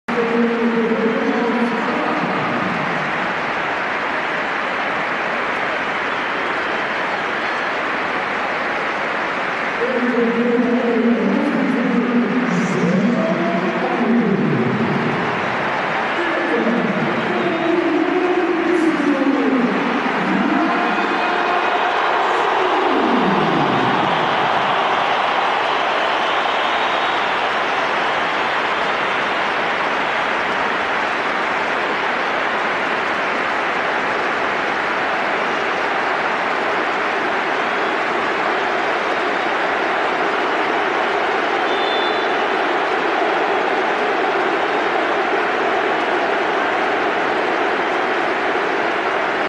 JOGADOR UCRANIANO DO BENFICA É APLAUDIDO DE PÉ PELA TORCIDA EM LISBOA E, EMOCIONADO, CHORA EM CAMPO
O atacante ucraniano Roman Yaremchuk, do Benfica, foi aplaudido de pé pelos torcedores do seu clube ao ingressar no lugar de Darwin Nuñez em partida pelo Campeonato Português contra o Vitória de Guimarães. Vídeo mostra o jogador às lágrimas com a homenagem